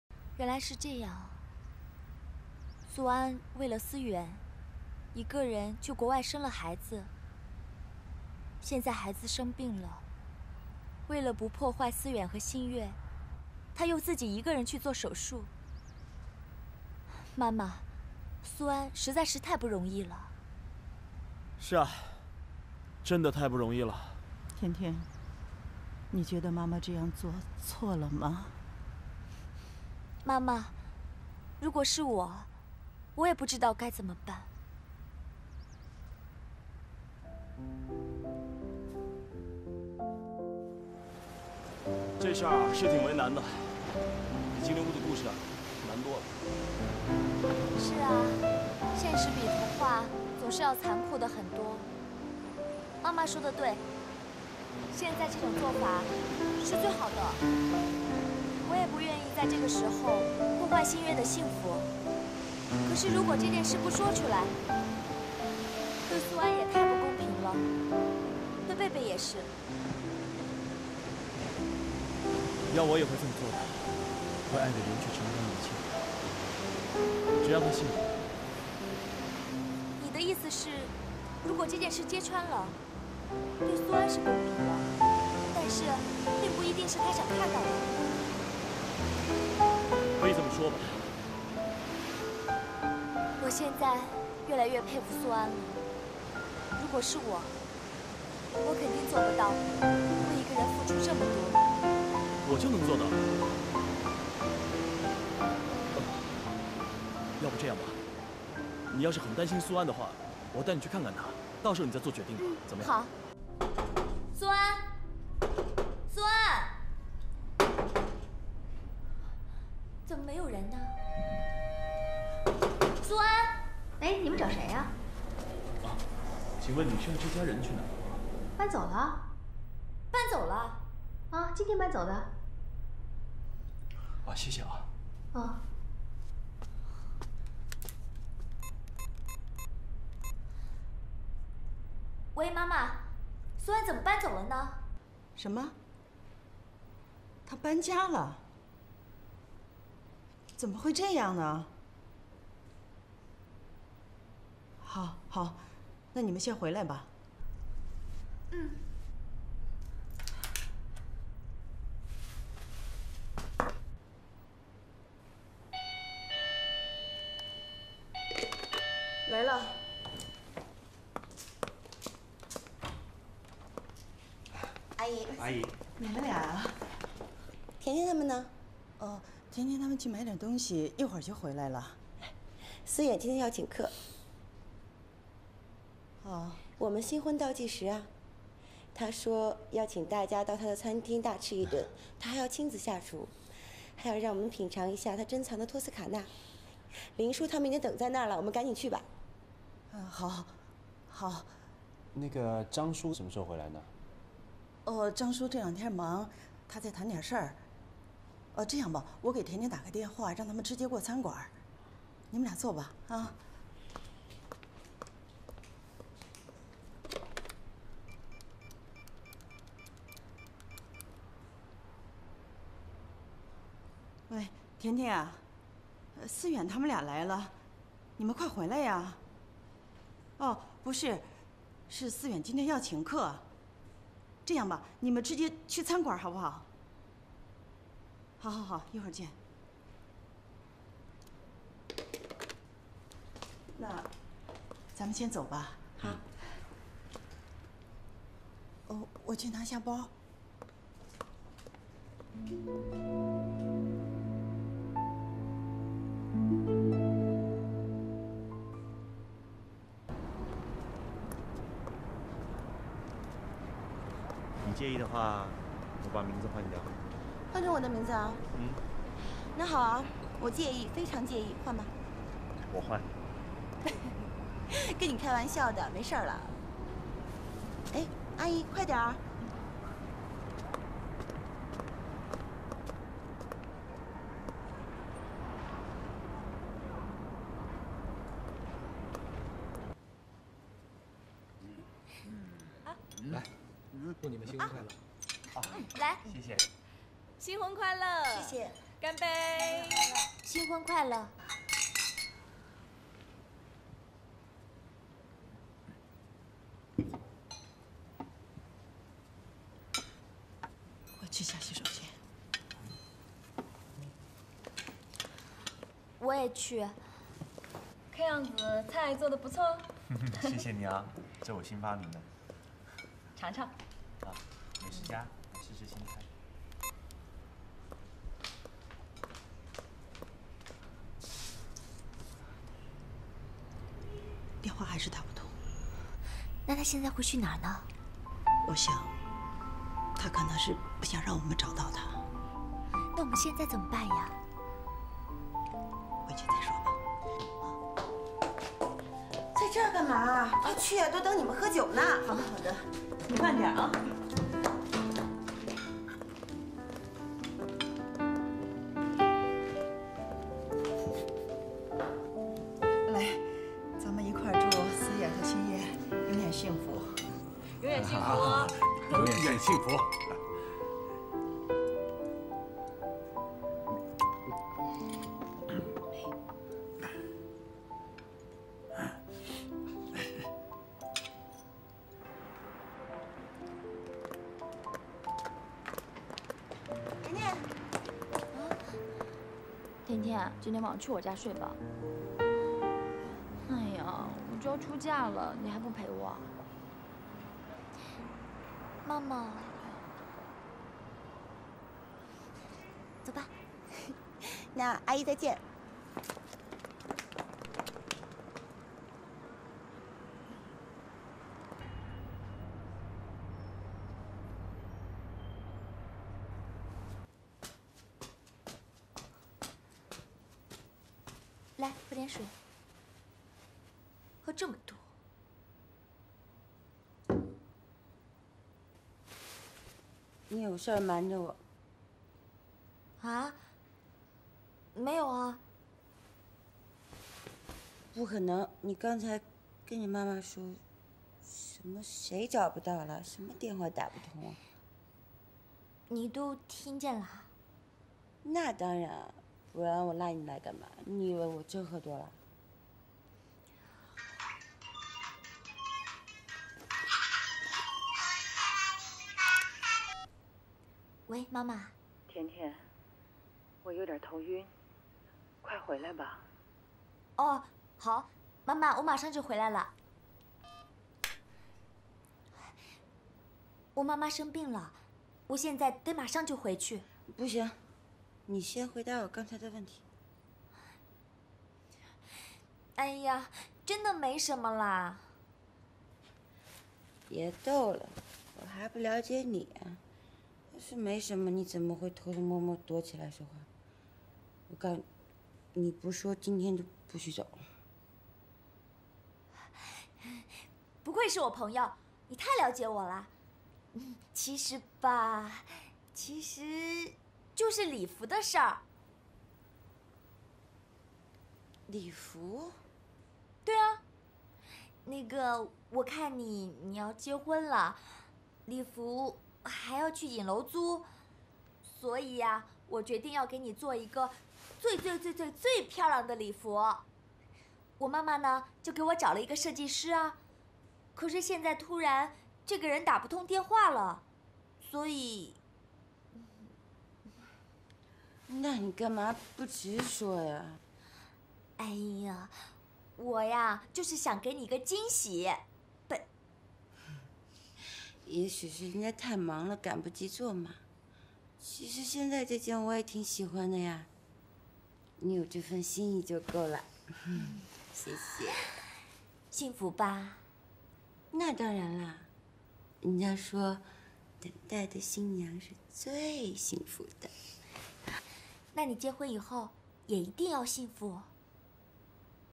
前夫馬上要結婚，前妻為不打擾他的生活，選擇帶孩子離開！ep18
每日更新電影精彩片段，別忘了訂閱關注奧！